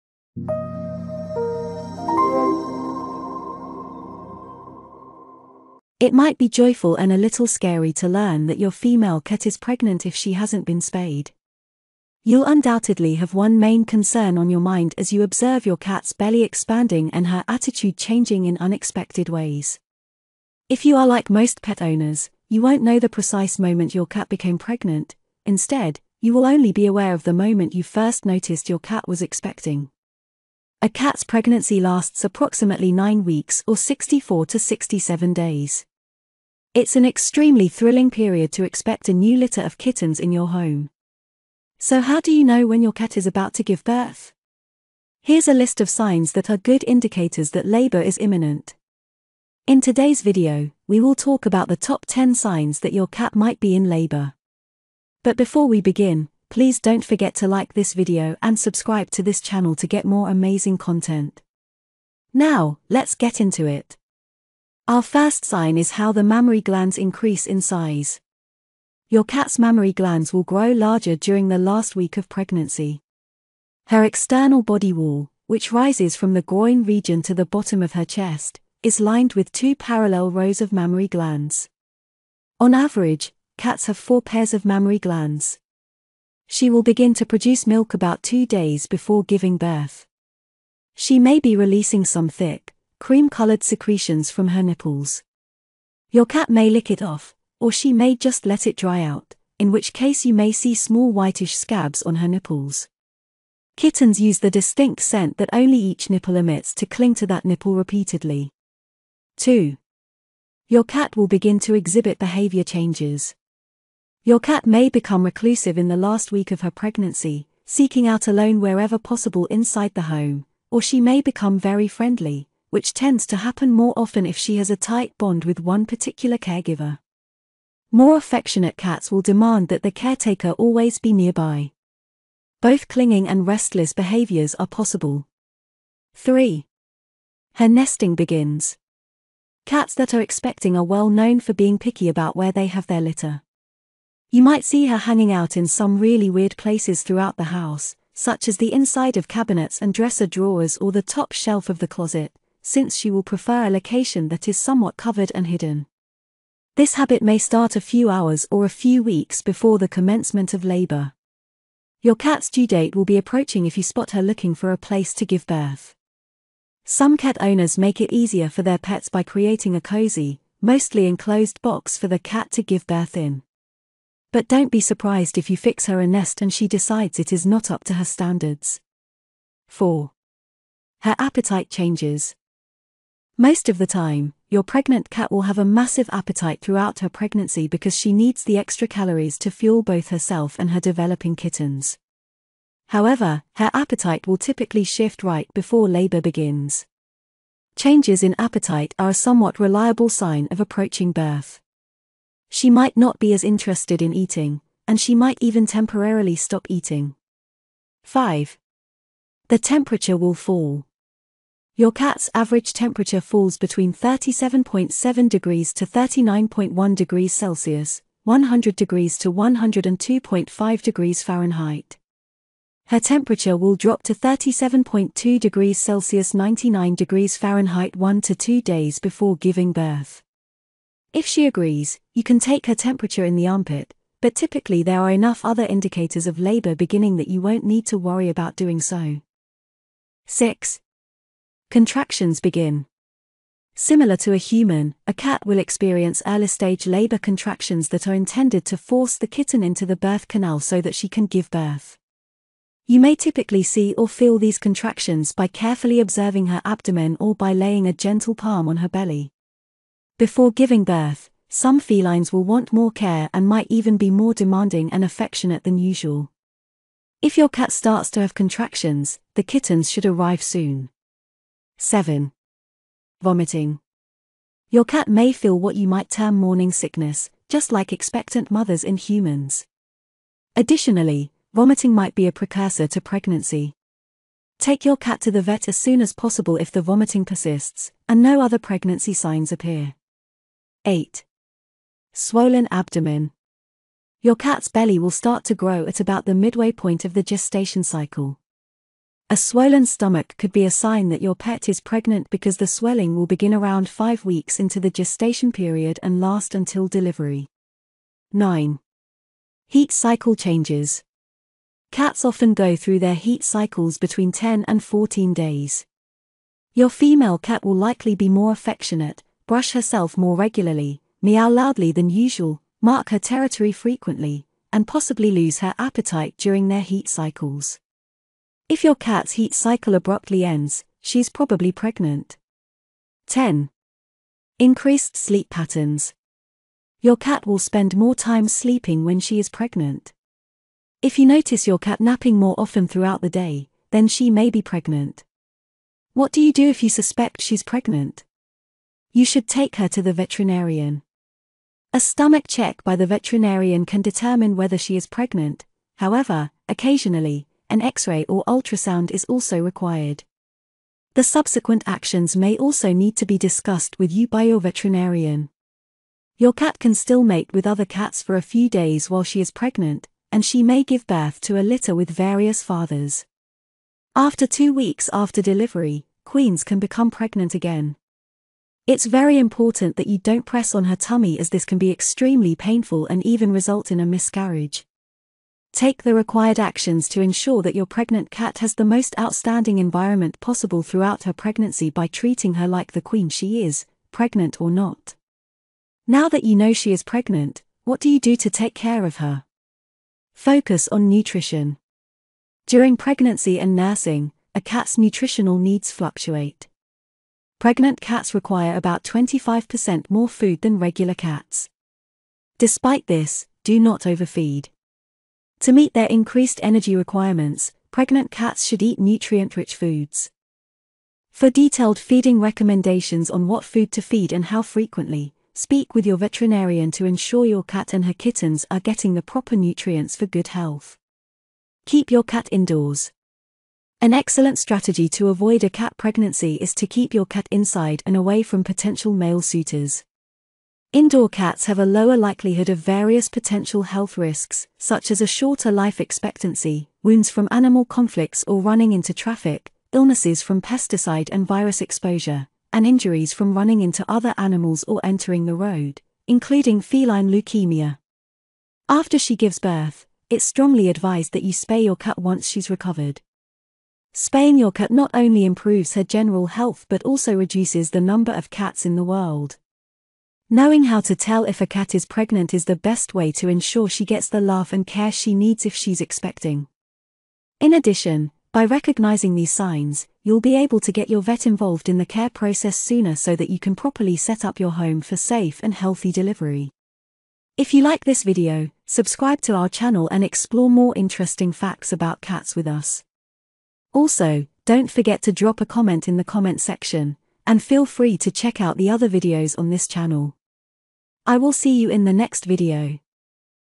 Top 10 Signs Your Cat is in LABOR
How do you know if your cat is going into labor?

A cat's pregnancy lasts approximately 9 weeks or 64 to 67 days. It's an extremely thrilling period to expect a new litter of kittens in your home. So how do you know when your cat is about to give birth?

Here’s a list of signs that are good indicators that labor is imminent! In today’s video, we will talk about the top 10 signs that your cat might be in labor.

Our first sign is how the mammary glands increase in size.

Your cat's mammary glands will grow larger during the last week of pregnancy. Her external body wall, which rises from the groin region to the bottom of her chest, is lined with two parallel rows of mammary glands. On average, cats have four pairs of mammary glands. She will begin to produce milk about 2 days before giving birth. She may be releasing some thick, cream-colored secretions from her nipples. Your cat may lick it off, or she may just let it dry out, in which case you may see small whitish scabs on her nipples. Kittens use the distinct scent that only each nipple emits to cling to that nipple repeatedly.

Two. Your cat will begin to exhibit behavior changes.

Your cat may become reclusive in the last week of her pregnancy, seeking out alone wherever possible inside the home, or she may become very friendly, which tends to happen more often if she has a tight bond with one particular caregiver. More affectionate cats will demand that the caretaker always be nearby. Both clinging and restless behaviors are possible.

Three. Her Nesting begins.

Cats that are expecting are well known for being picky about where they have their litter. You might see her hanging out in some really weird places throughout the house, such as the inside of cabinets and dresser drawers or the top shelf of the closet since she will prefer a location that is somewhat covered and hidden. This habit may start a few hours or a few weeks before the commencement of labor. Your cat's due date will be approaching if you spot her looking for a place to give birth. Some cat owners make it easier for their pets by creating a cozy, mostly enclosed box for the cat to give birth in. But don't be surprised if you fix her a "nest" and she decides it is not up to her standards.

Four. Her appetite changes.

Most of the time, your pregnant cat will have a massive appetite throughout her pregnancy because she needs the extra calories to fuel both herself and her developing kittens. However, her appetite will typically shift right before labor begins. Changes in appetite are a somewhat reliable sign of approaching birth. She might not be as interested in eating, and she might even temporarily stop eating.

Five. The temperature will fall.

Your cat’s average temperature falls between 37.7º to 39.1ºC (100º to 102.5ºF). Her temperature will drop to 37.2ºC (99ºF) one to two days before giving birth. If she agrees, you can take her temperature in the armpit, but typically there are enough other indicators of labor beginning that you won't need to worry about doing so.

Six. Contractions begin.

Similar to a human, a cat will experience early-stage labor contractions that are intended to force the kitten into the birth canal so that she can give birth. You may typically see or feel these contractions by carefully observing her abdomen or by laying a gentle palm on her belly. Before giving birth, some felines will want more care and might even be more demanding and affectionate than usual. If your cat starts to have contractions, the kittens should arrive soon.

Seven. Vomiting.

Your cat may feel what you might term morning sickness, just like expectant mothers in humans. Additionally, vomiting might be a precursor to pregnancy. Take your cat to the vet as soon as possible if the vomiting persists, and no other pregnancy signs appear.

Eight. Swollen Abdomen.

Your cat's belly will start to grow at about the midway point of the gestation cycle. A swollen stomach could be a sign that your pet is pregnant because the swelling will begin around five weeks into the gestation period and last until delivery.

Nine. Heat cycle changes.

Cats often go through their heat cycles between 10 and 14 days. Your female cat will likely be more affectionate, brush herself more regularly, meow loudly than usual, mark her territory frequently, and possibly lose her appetite during their heat cycles. If your cat's heat cycle abruptly ends, she's probably pregnant.

Ten. Increased sleep patterns.

Your cat will spend more time sleeping when she is pregnant. If you notice your cat napping more often throughout the day, then she may be pregnant.

What do you do if you suspect she's pregnant?